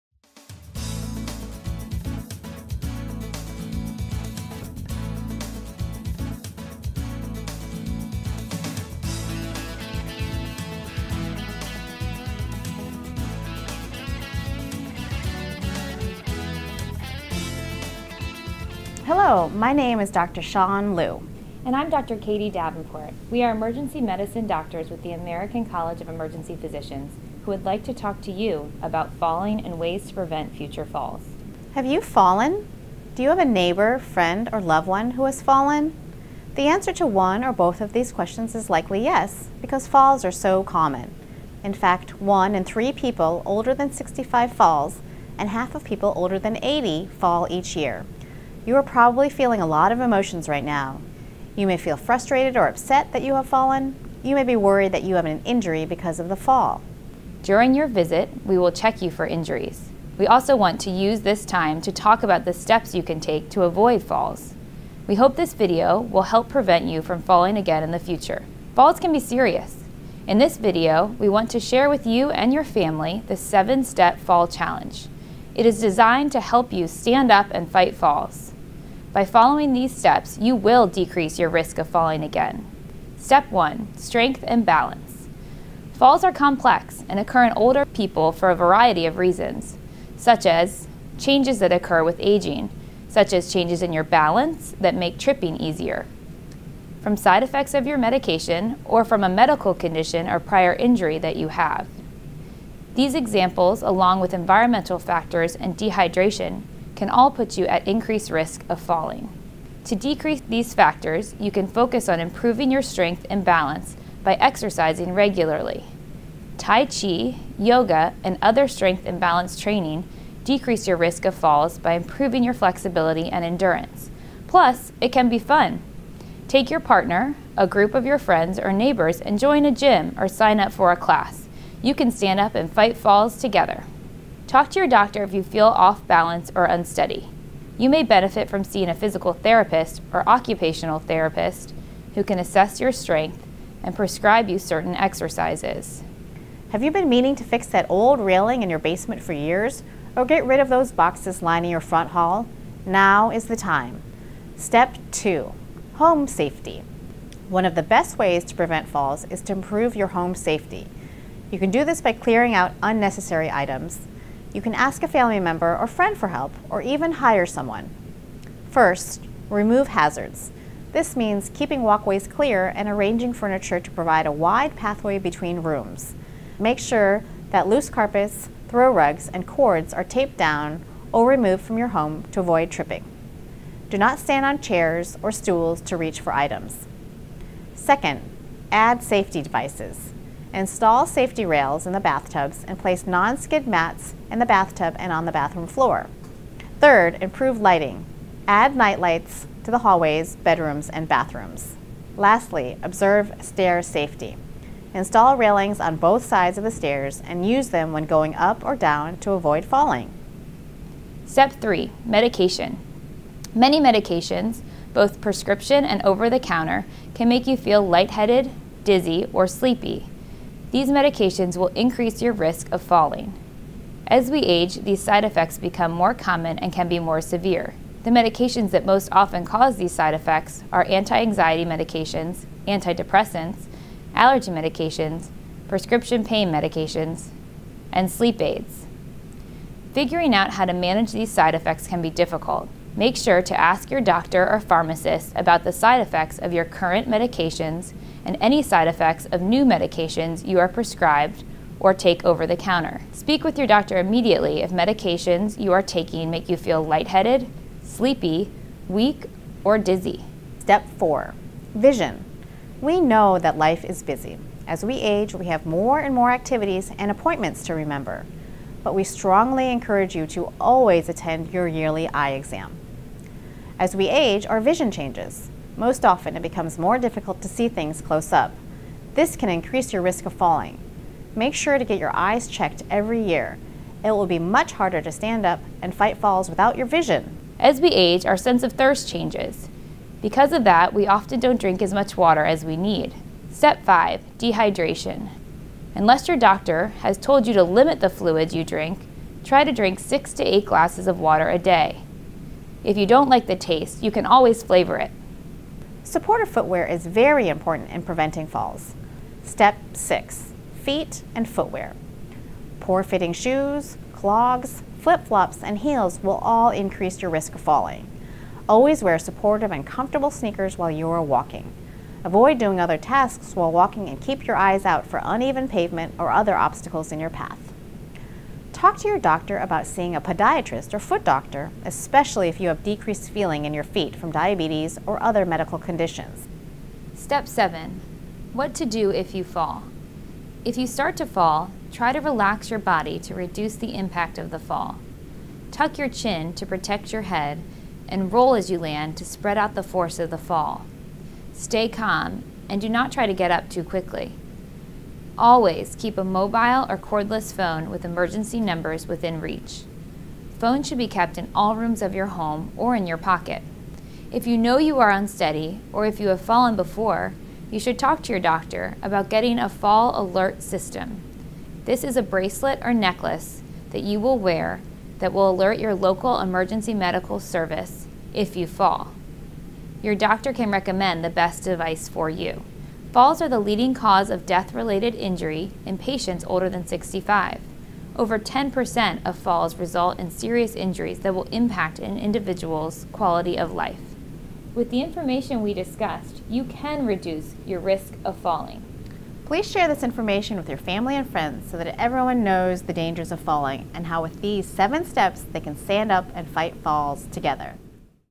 7 Step Fall Challenge
Falls can be serious events, even fatal ones.  One in 3 people over 65 years of age fall each year.  The number increases as you get older.  Drs. Shan Liu and Katie Davenport, who are emergency physicians, walk you through how to avoid falls and what to do in case of a fall.